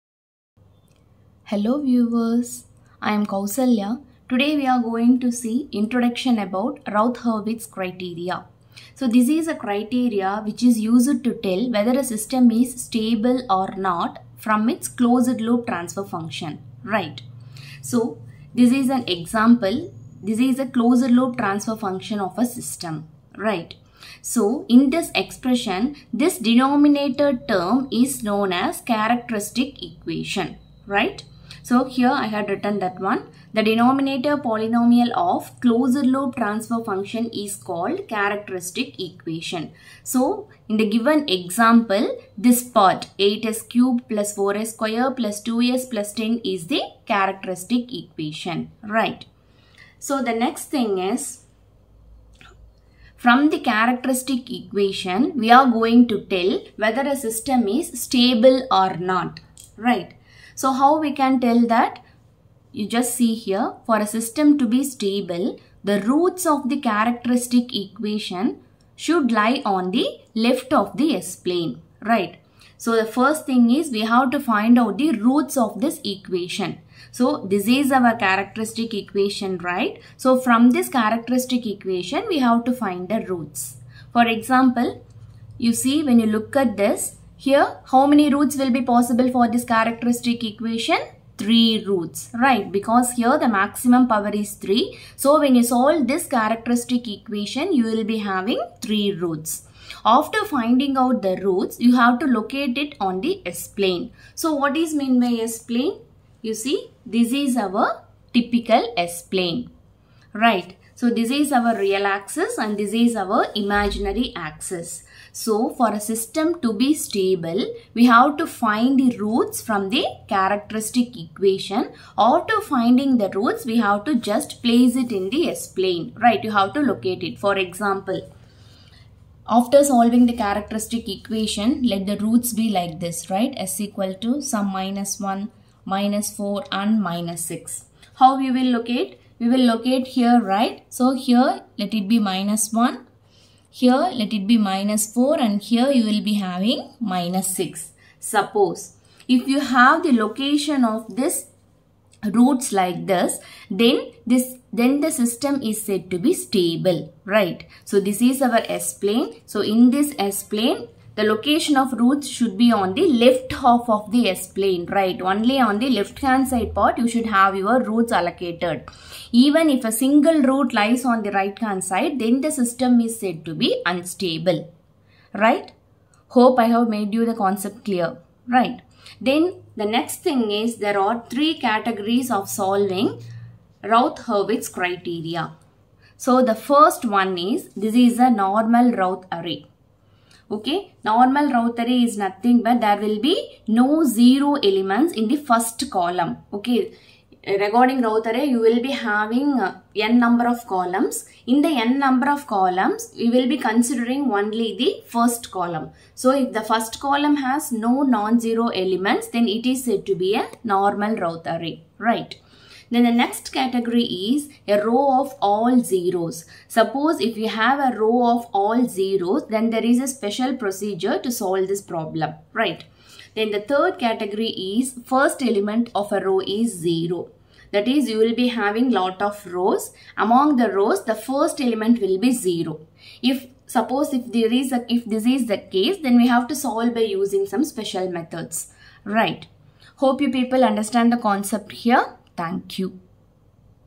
Introduction to Routh Hurwitz Criteria
All the three cases are explained.characteristic equation of the system is discussed.S-plane  introduction is given.